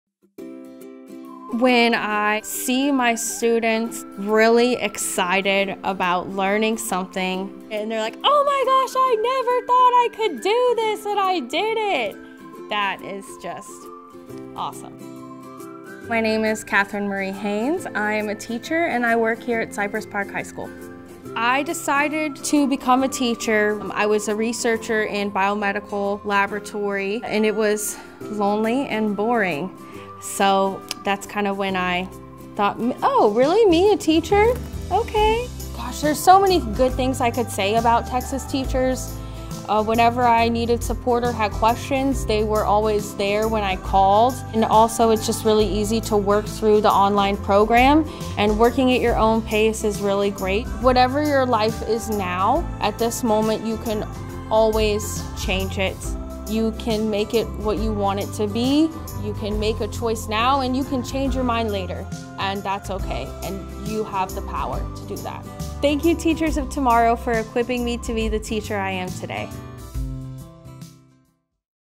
Teacher of the Year: Kathryn Haynes
As 2020 Texas Teacher of the Year, I am proud to say, thank you, Teachers of Tomorrow, for helping me get into the career that has changed my life forever. The entire program was user-friendly, self-paced, and helped me change. Thank you to my Field Supervisor for believing in me.